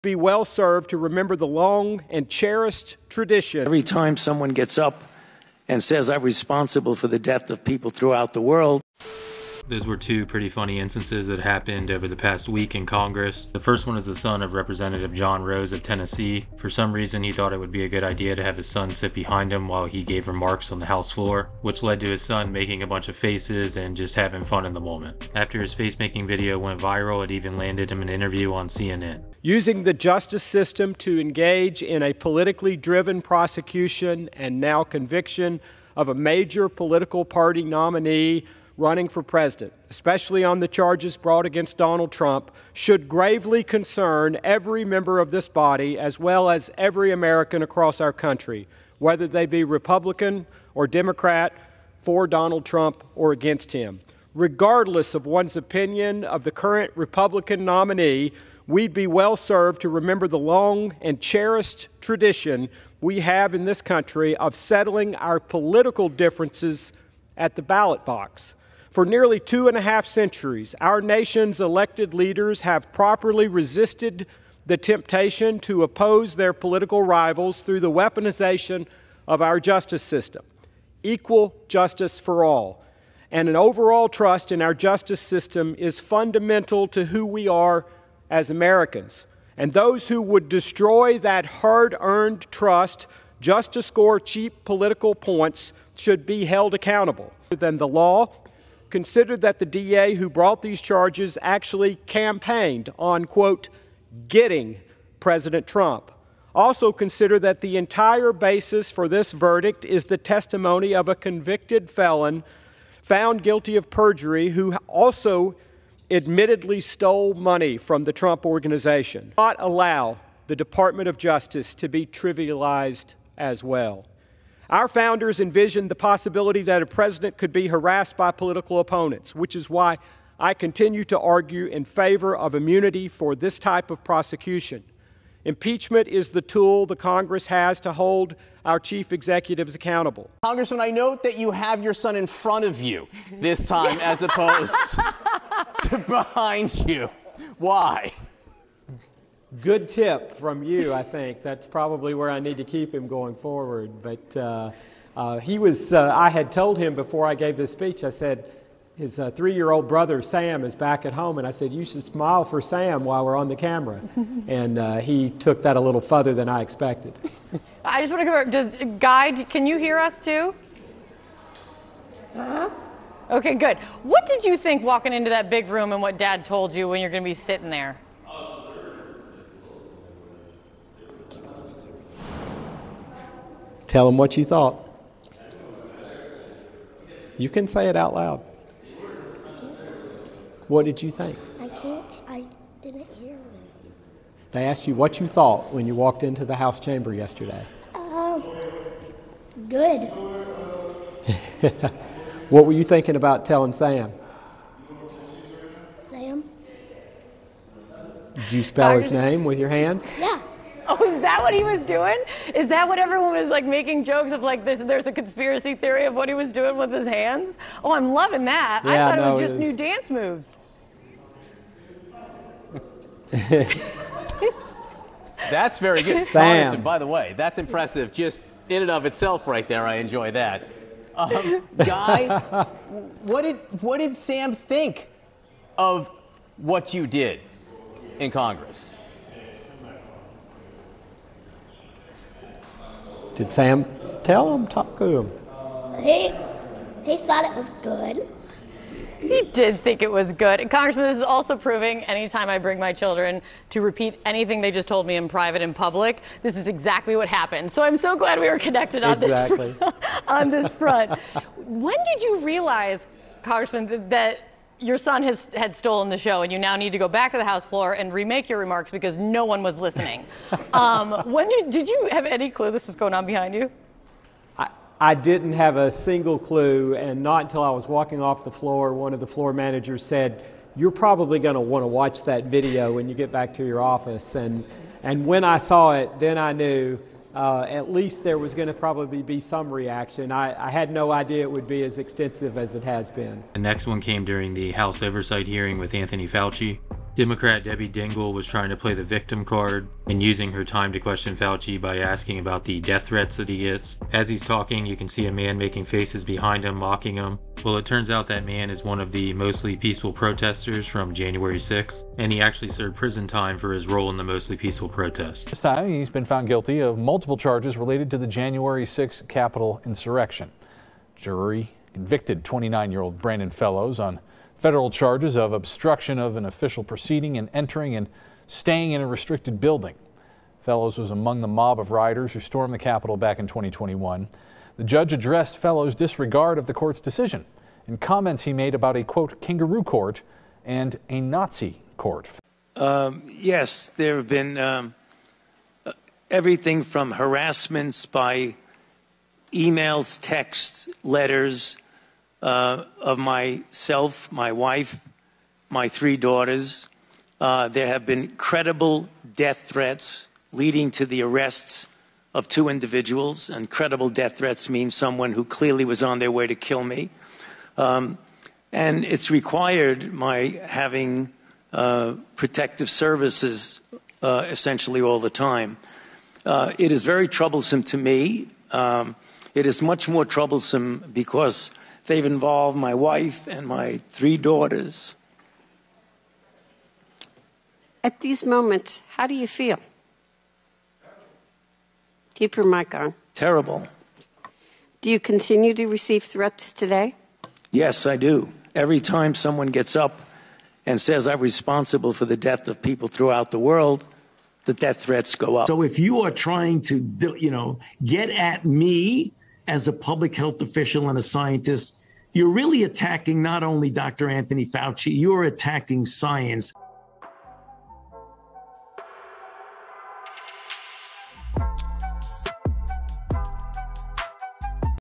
Congressional Trolling
The son of Rep. John Rose (TN-6th) makes faces on the House floor and a man makes faces behind Anthony Fauci during a House Oversight Committee hearing